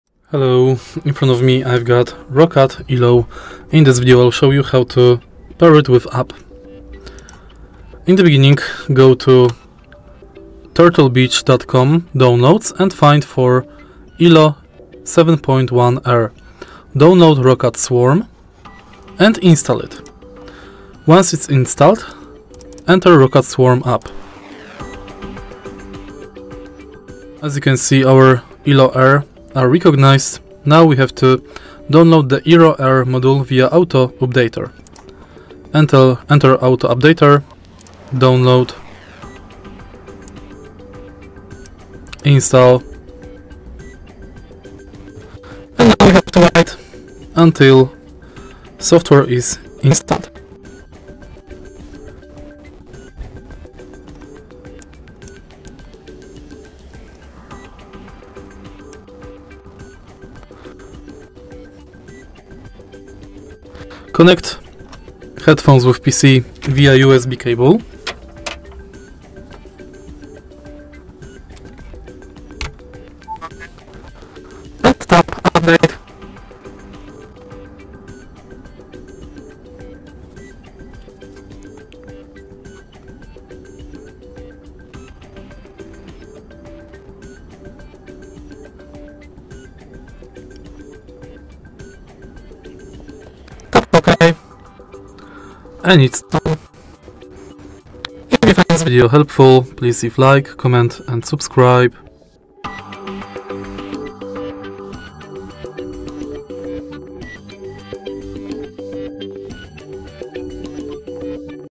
How to Pair Roccat Elo 7.1 with App? | Manage App | Roccat Swarm
Unlock the full potential of your Roccat Elo 7.1 headset by pairing it with the Roccat Swarm app. In this video, we’ll guide you through the steps to connect and manage your headset using the app, ensuring you get the best gaming and audio experience.

In this video, you’ll learn:

Downloading and Installing Roccat Swarm:
Where to find the Roccat Swarm app for download.
Step-by-step installation process on your PC.
Connecting Roccat Elo 7.1 to the App:
Ensuring your headset is properly connected to your PC.
Opening the Roccat Swarm app and detecting your Roccat Elo 7.1 headset.
Managing Settings in Roccat Swarm:
Adjusting audio settings, such as equalizer, surround sound, and volume levels.
Customizing lighting effects and macros.
Saving and applying profiles for different games or activities.
Troubleshooting Common Issues:
What to do if your headset isn’t detected by the app.
How to update firmware and drivers.

FAQs:

What should I do if Roccat Swarm doesn’t recognize my headset?
Can I create custom audio profiles for different games?
How do I update my headset’s firmware using the app?
Enhance your Roccat Elo 7.1 experience by leveraging the powerful features of the Roccat Swarm app.